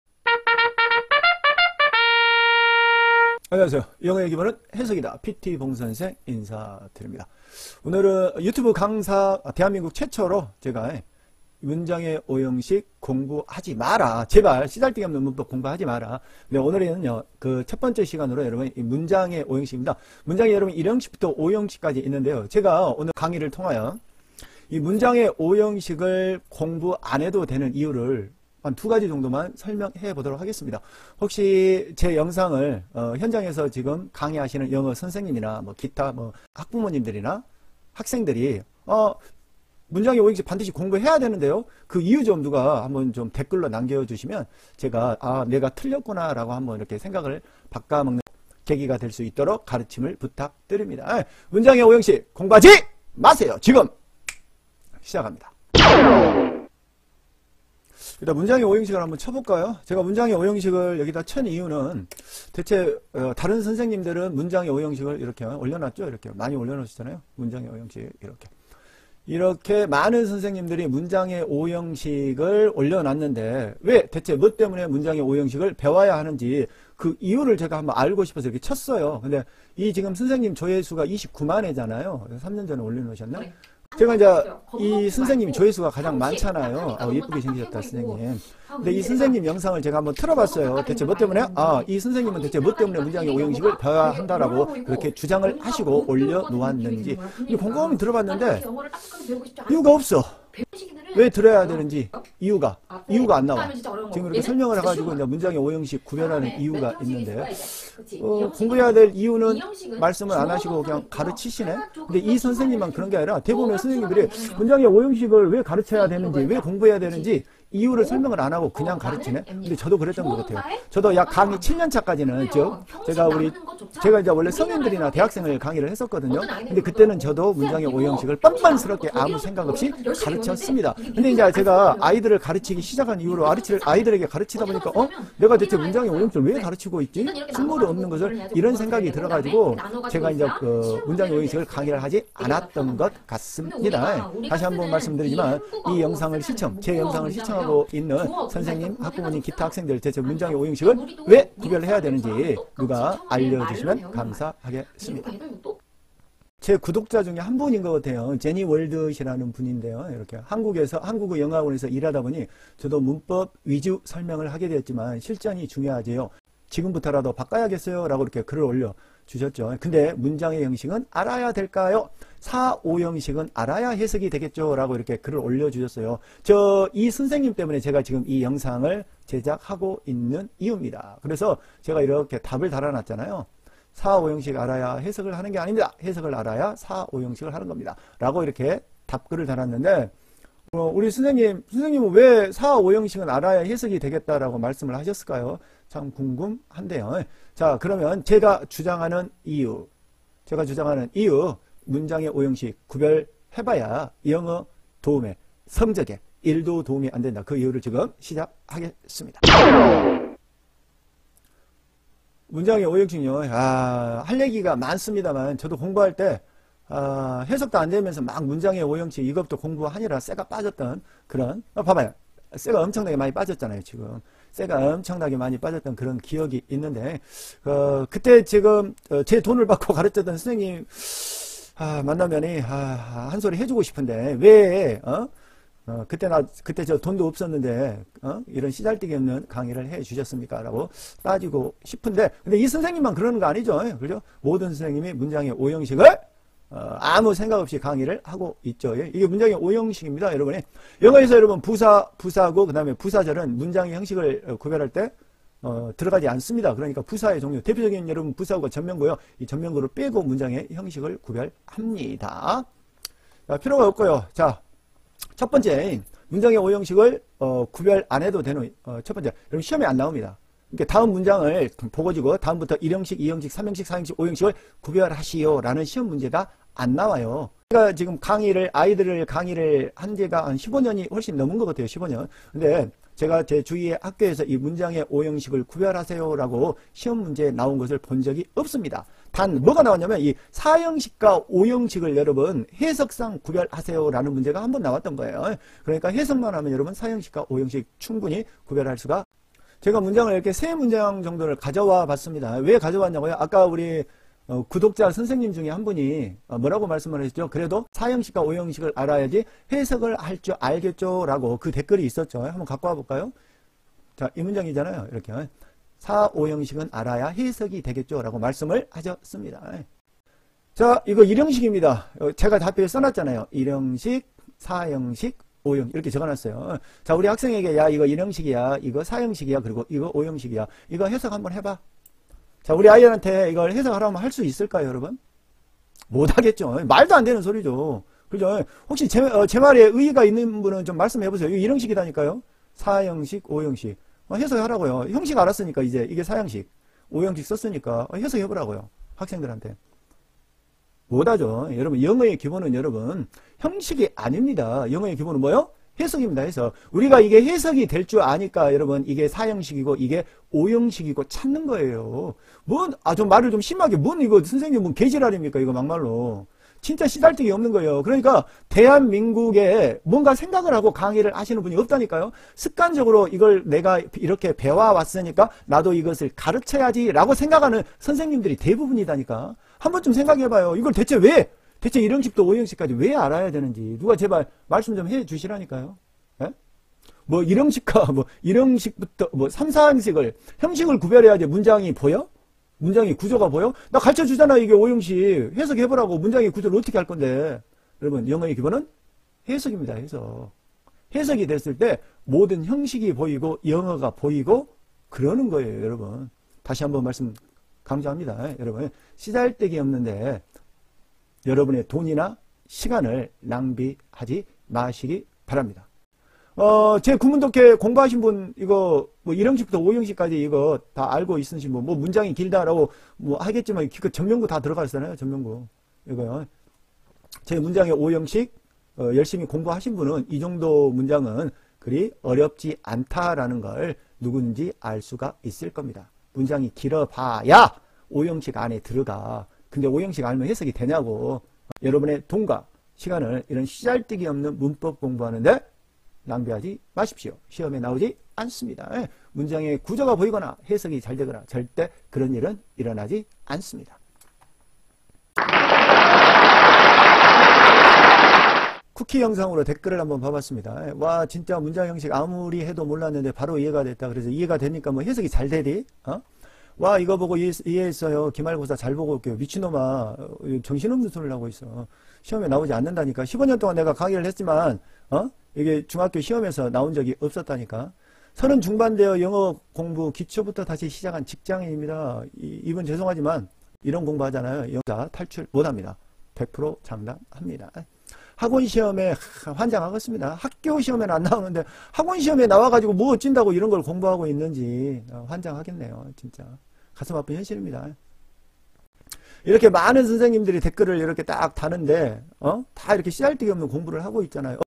시간과 돈을 버리는 5형식 구별강의 - 문장의 5형식 구별공부하지 마세요.
저도 강의 7-8년차 까지는 문장의 5형식 구별을 을 설명했습니다.
왜 설명했을까요??
이유는 단한가지 내가 그렇게 배워왔으니까!   - 얼마나 어릭석은 행동입니까